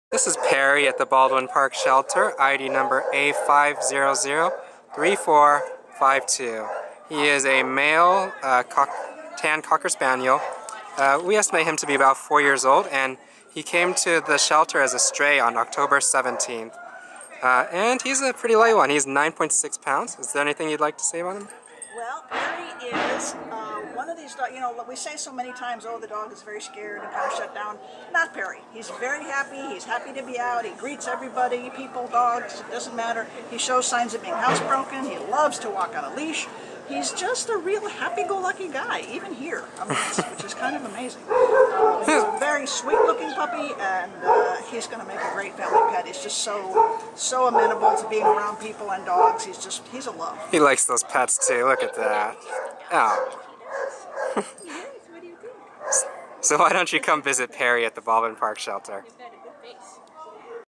A5003452 Perry | Cocker Spaniel Mix
A5003452 Perry is a chipper 4-year-old tan-and-white male Cocker Spaniel mix who came to the Baldwin Park Animal Care Center on October 17th as a stray from Pomona. Weighing 10 lbs, Perry is a real “Disney dog,” with heart-melting loving eyes and gorgeous palomino coloring. He’s just as sweet and cuddly as he looks -- he’s friendly as can be. Perry trots beautifully on leash, shows signs of housebreaking, and gets along well with his kennel mates. He takes quickly to new people and leaves his kennel willingly with a handler. We sometimes describe a dog as a “plug-and-play” pup -- one who will fit perfectly into any household, right off the bat. Perry is the very definition of plug-and-play. He’ll make a great indoor pet and companion for just about any household.